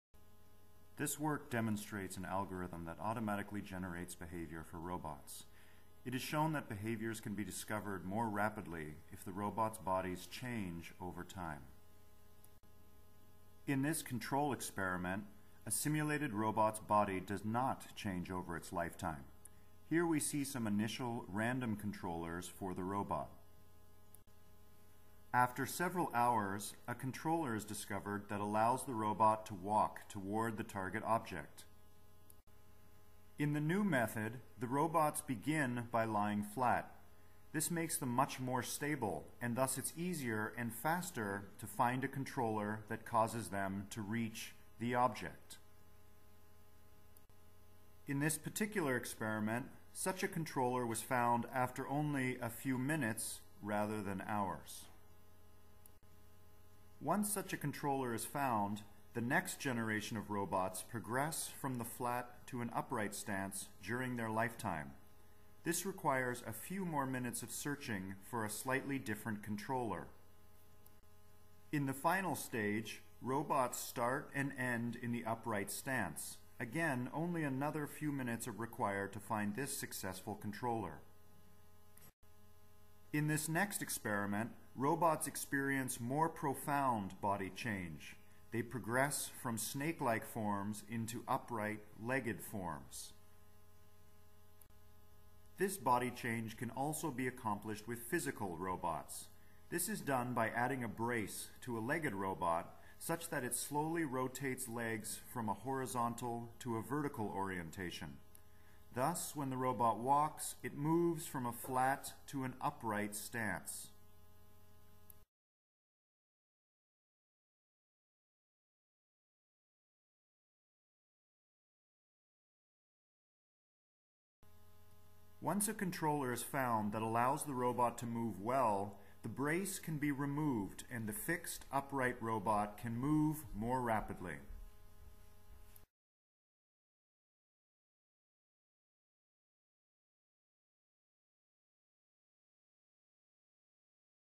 Morphological change in autonomous machines
This research demonstrates that robots which change their bodies during their lifetime lead to legged walking machines faster than if their bodies do not change.